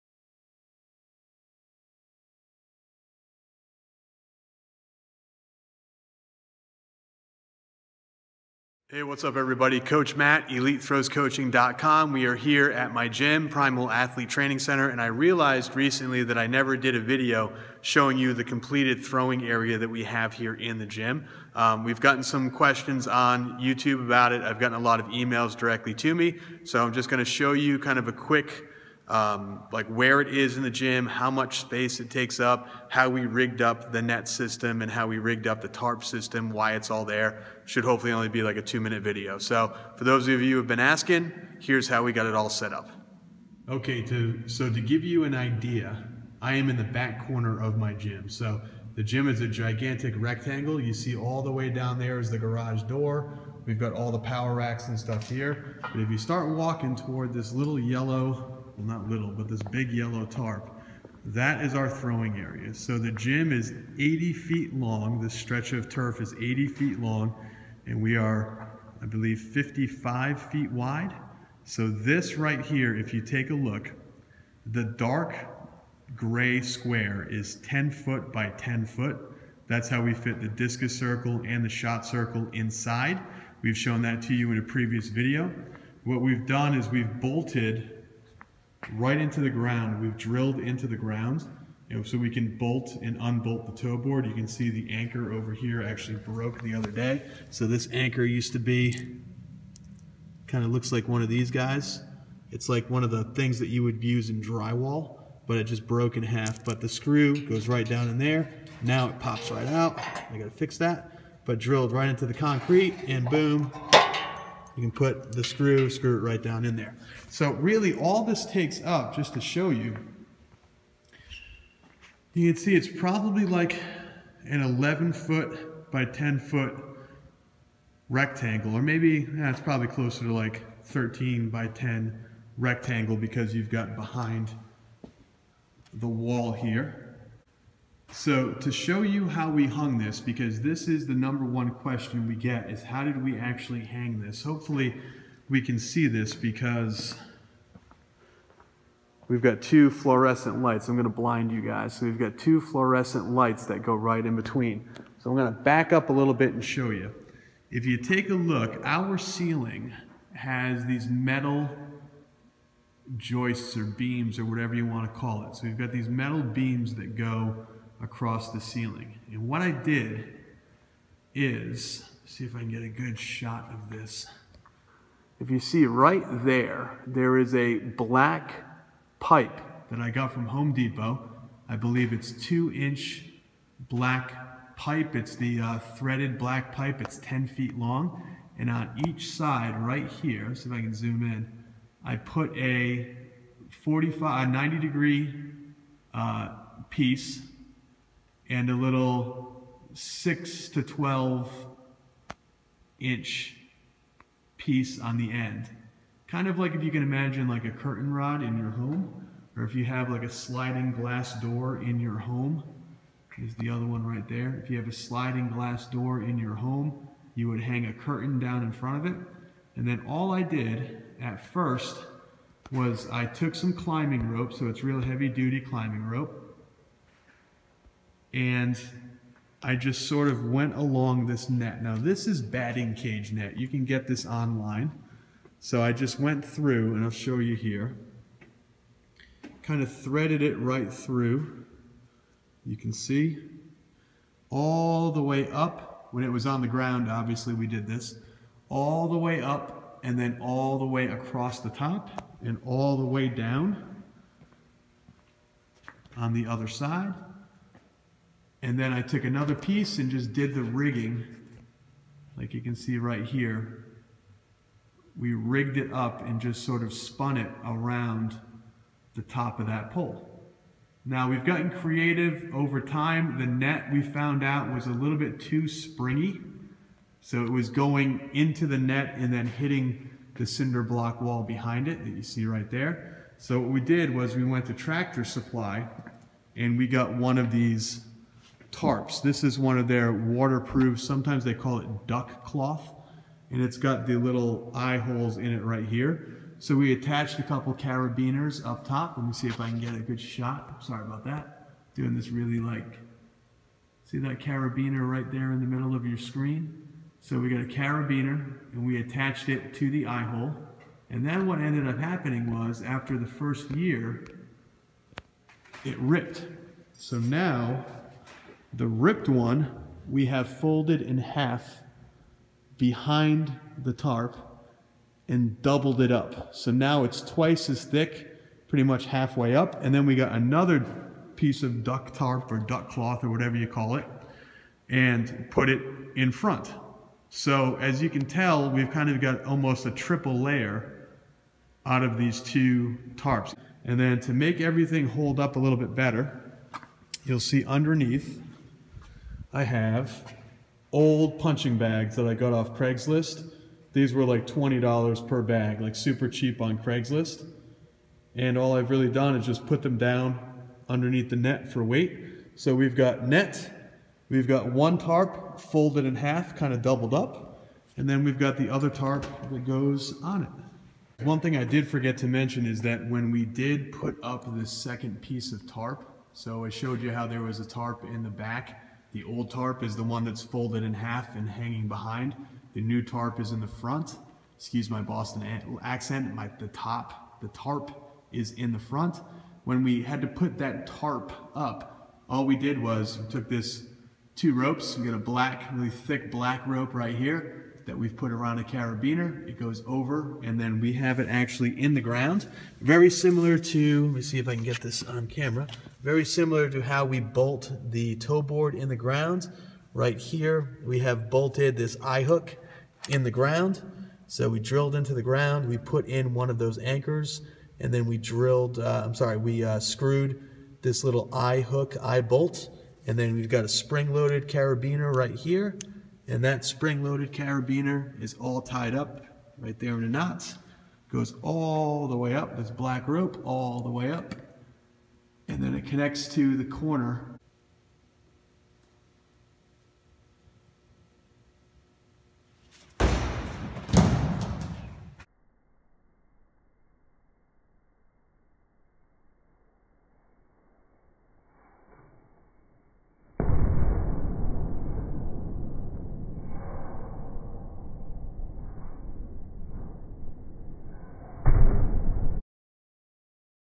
Part 4 - Indoor Throwing Circle and Net System
A few years back, I did a video series showing how we measured, painted, and installed our toe board on the indoor throwing are at the gym. However, I never showed anything about the tarp and net system. So here you go!